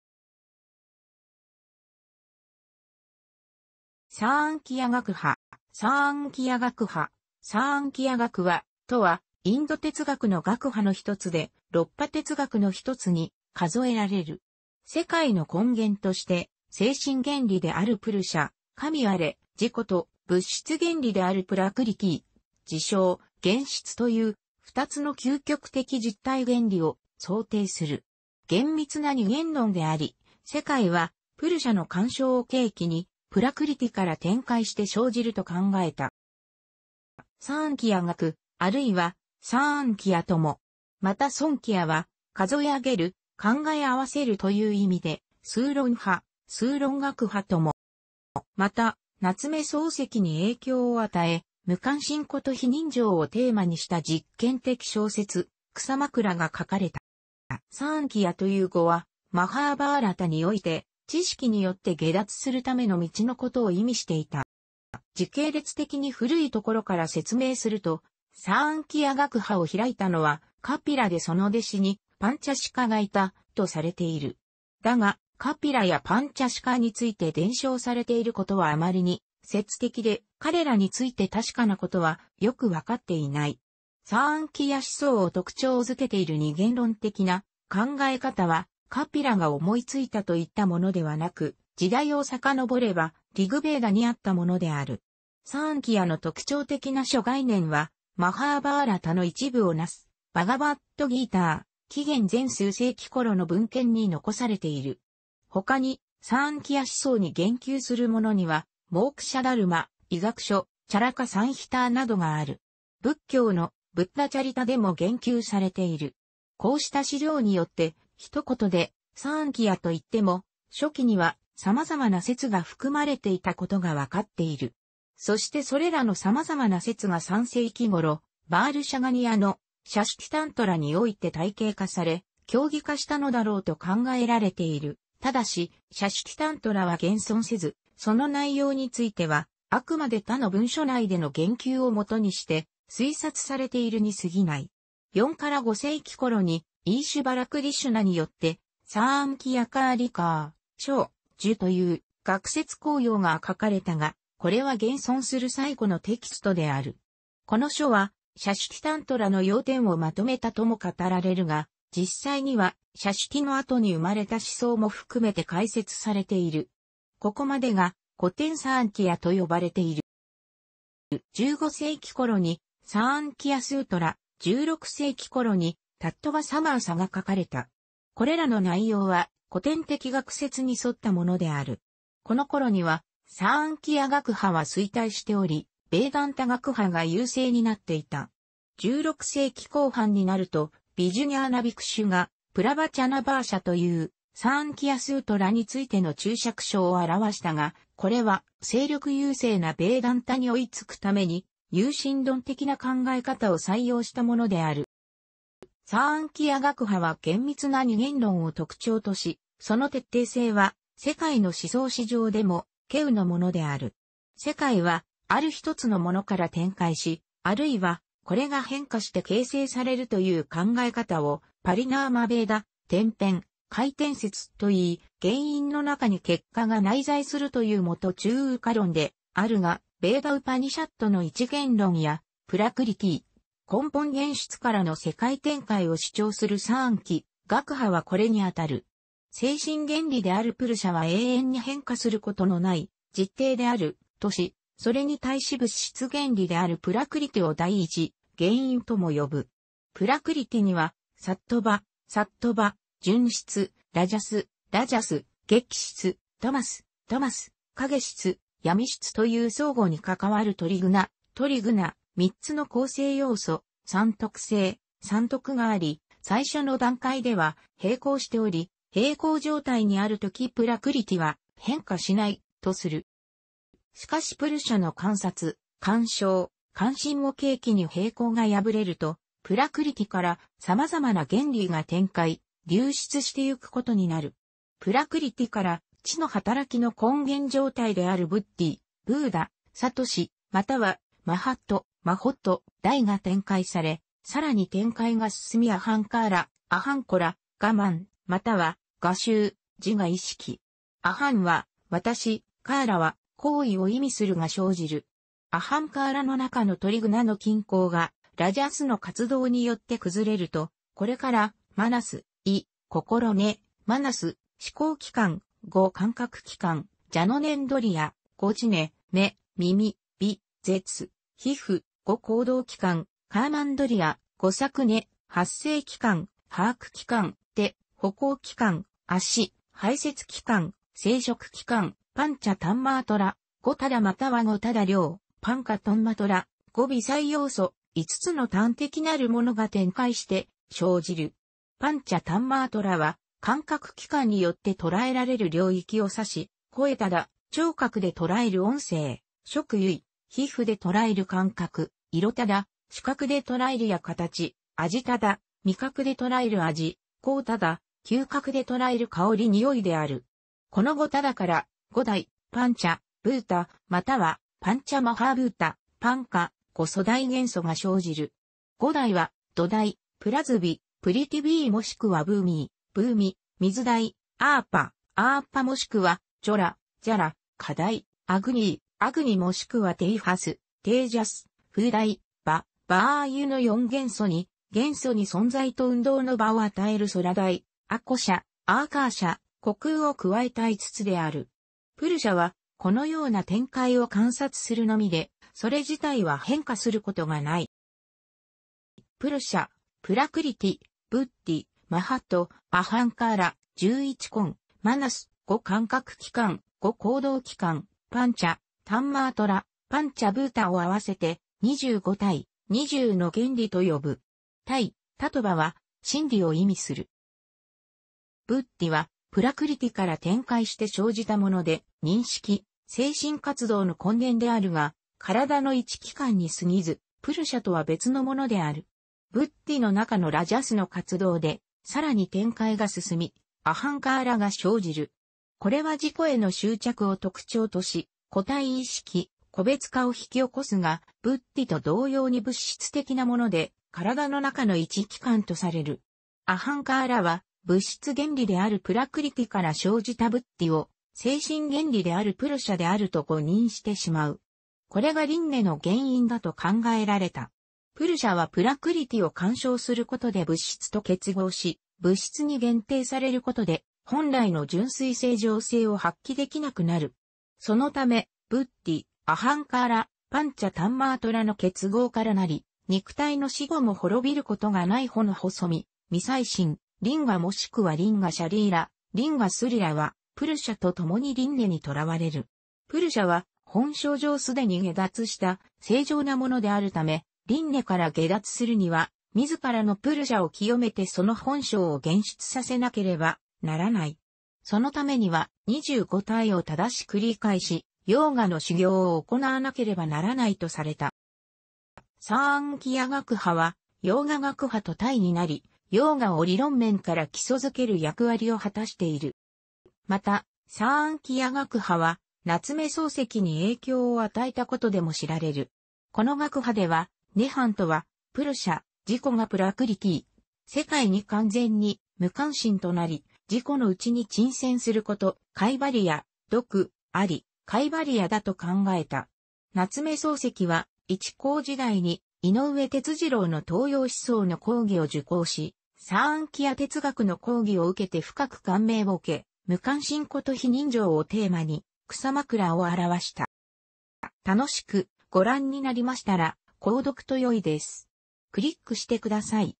サーンキヤ学派
サーンキヤ学派（サーンキヤがくは、）とは、インド哲学の学派のひとつで、六派哲学の1つに数えられる。世界の根源として、精神原理であるプルシャ（神我、自己）と物質原理であるプラクリティ（自性、原質）という、2つの究極的実体原理を想定する。厳密な二元論であり、世界はプルシャの観照を契機に、プラクリティから展開して生じると考えた。

サーンキヤ学、あるいはサーンキヤとも。またSāṅkhya(サーンキヤ)は「数え上げる」「考え合わせる」という意味で、数論派、数論学派とも。

また、夏目漱石に影響を与え、無関心こと非人情をテーマにした実験的小説『草枕』が書かれた。

「サーンキヤ」という語は『マハーバーラタ』において、知識によって解脱するための道のことを意味していた。

時系列的に古いところから説明すると、サーンキヤ学派を開いたのはカピラでその弟子にパンチャシカがいた、とされている。だがカピラやパンチャシカについて伝承されていることはあまりに伝説的で、彼らについて確かなことはよく分かっていない。サーンキヤ思想を特徴づけている二元論的な考え方は、カピラが思いついたといったものではなく、時代を遡れば『リグ・ヴェーダ』にあったものである。サーンキヤの特徴的な諸概念は『マハーバーラタ』の一部をなす『バガヴァッド・ギーター』（紀元前数世紀ころの文献）に残されている。他に、サーンキヤ思想に言及するものには「モークシャ・ダルマ」、医学書『チャラカ・サンヒター』などがある。仏教の『ブッダチャリタ』でも言及されている。こうした資料によって、ひとことでサーンキヤと言っても、初期には様々な説が含まれていたことが判っている。そしてそれらの様々な説が3世紀ごろ、ヴァールシャガニヤの『シャシュティ・タントラ』(、六十科論)において体系化され、教義化したのだろうと考えられている（ただし、『シャシュティ・タントラ』は現存せず、その内容については、あくまで他の文書内での言及をもとにして推察されているにすぎない）。4～5世紀ころに、イーシュヴァラクリシュナによって『サーンキヤ・カーリカー（頌、じゅ）』という、学説綱要が書かれたが、これは現存する最古のテキストである。この書は「『シャシュティ・タントラ』の要点をまとめた」とも語られるが、実際には『シャシュティ～』の後に生まれた思想も含めて解説されている。ここまでが「古典サーンキヤ」と呼ばれている。

15世紀ころに『サーンキヤ・スートラ』、16世紀ころに『タットヴァ・サマーサ』が書かれた。これらの内容は古典的学説に沿ったものである。このころには、サーンキヤ学派は衰退しており、ヴェーダンタ学派が優勢になっていた。16世紀後半になるとヴィジュニャーナビクシュが『プラヴァチャナ・バーシャ』という、『サーンキヤ・スートラ』についての注釈書を著したが、これは勢力優勢なヴェーダンタに追いつくために有神論的な考え方を採用したものである。

サーンキヤ学派は厳密な二元論を特徴とし、その徹底性は世界の思想史上でも稀有のものである。世界はある一つのものから展開し、あるいはこれが変化して形成されるという考え方をパリナーマ・ヴェーダ（転変・開展説）といい、原因の中に結果が内在するという因中有果論であるが、ヴェーダ・ウパニシャッドの一元論や、プラクリティ（根本原質）からの世界展開を主張するサーンキヤ学派はこれにあたる。精神原理であるプルシャは永遠に変化することのない実体である、とし、それに対し物質原理であるプラクリティを第一原因とも呼ぶ。プラクリティには、サットヴァ（sattva/ सत्त्व 、純質）、ラジャス（Rajas/ रजस्、激質）、タマス（tamas/ तमस्、翳質・闇質）という相互に関わるトリ・グナ（tri-guṇa、3つの構成要素, 三特性、三徳）があり、最初の段階では平衡しており、平衡状態にあるときプラクリティは変化しない、とする。

しかしプルシャの観察（観照、関心）を契機に平衡が破れると、プラクリティから様々な原理が展開（流出）してゆくことになる。プラクリティから知の働きの根源状態であるブッディ（Buddhi, 覚）またはマハット（mahat, 大）が展開され、さらに展開が進みアハンカーラ（Ahaṅkāra, 我慢または我執, 自我意識。アハンは「私」、カーラは「行為」を意味する）が生じる。アハンカーラの中のトリ・グナの均衡がラジャスの活動によって崩れると、これからマナス（意, 心根、Manas、思考...